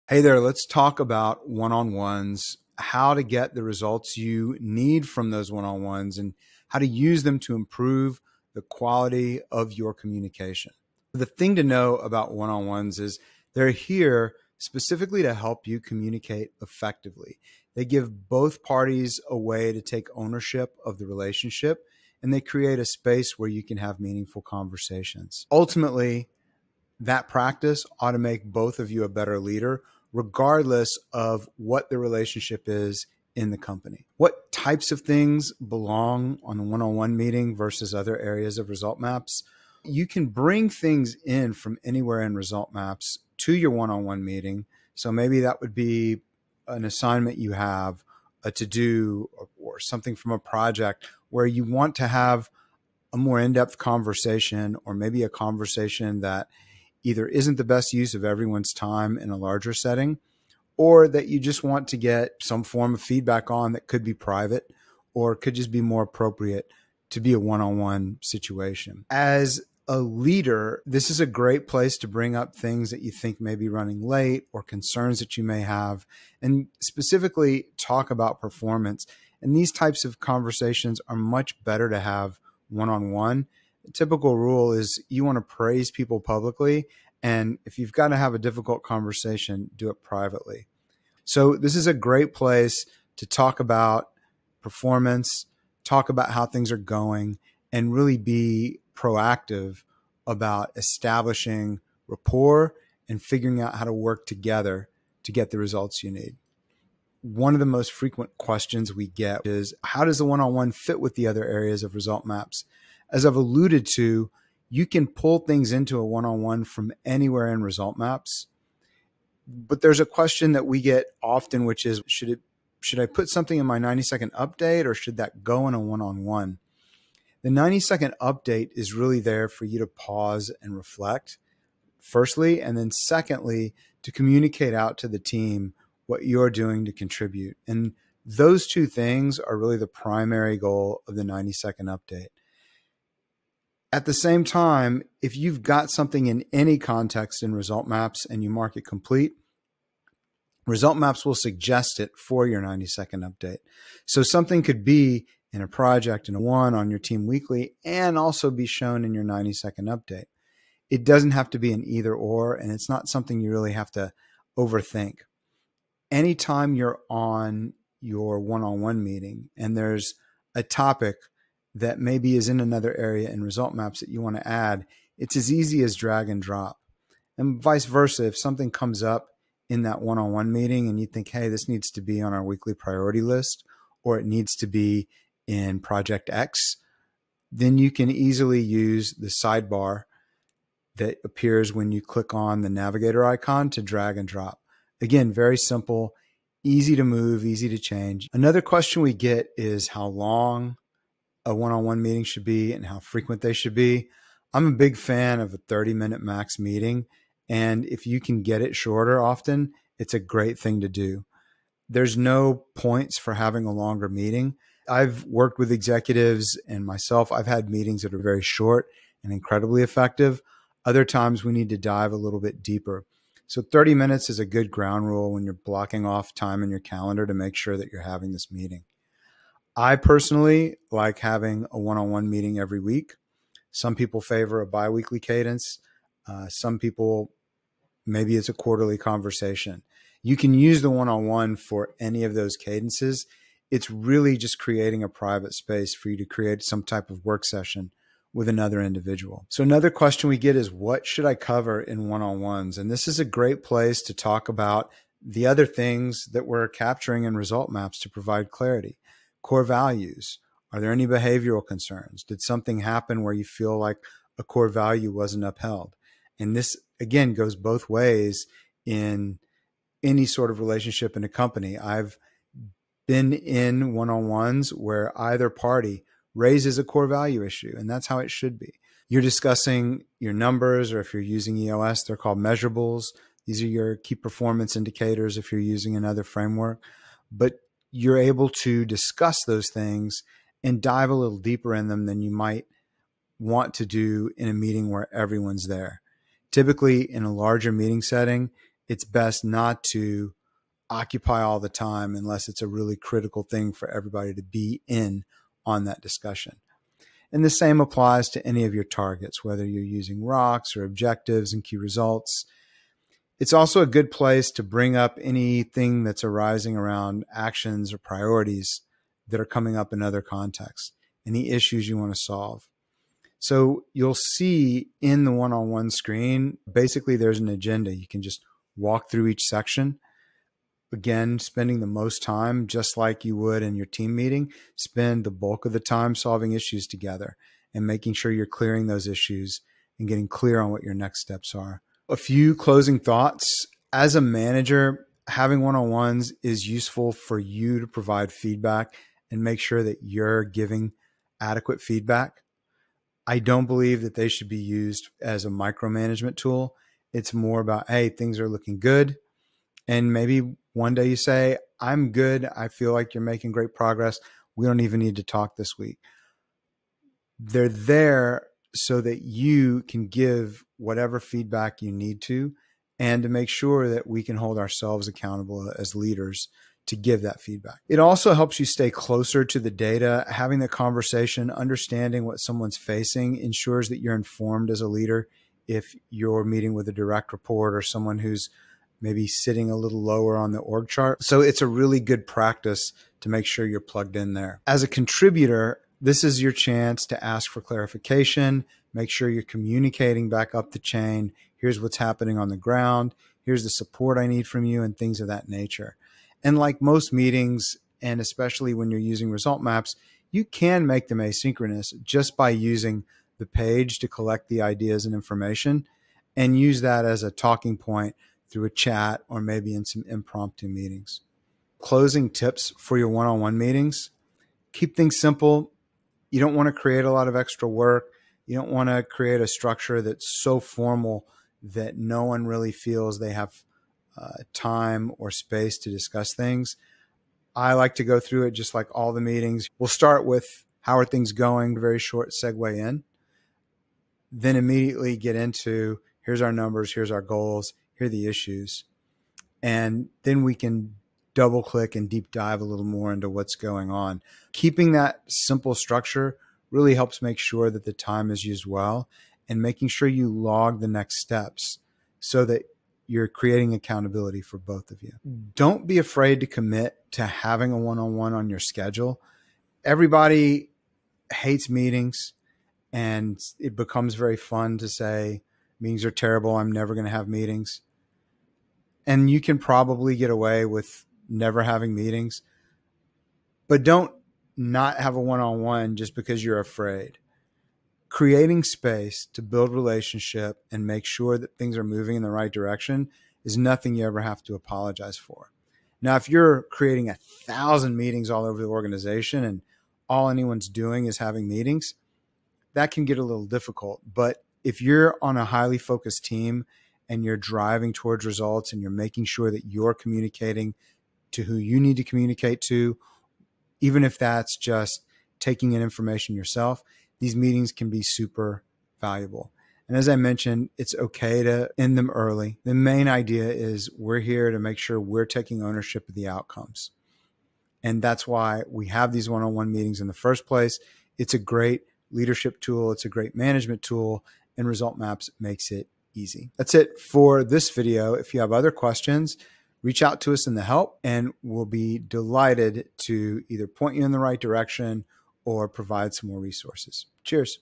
1x1 Tips From ResultMaps
This is a video about 1x1 Tips From ResultMaps

00:00 Introduction to Effective One on Ones
00:34 What to Discuss in One on Ones
01:46 Integrating One on Ones with Result Maps
03:18 Optimal Duration and Frequency of One on Ones
04:09 Key Topics to Cover in One on Ones
05:42 Final Thoughts and Best Practices
07:01 Closing Tips and Encouragement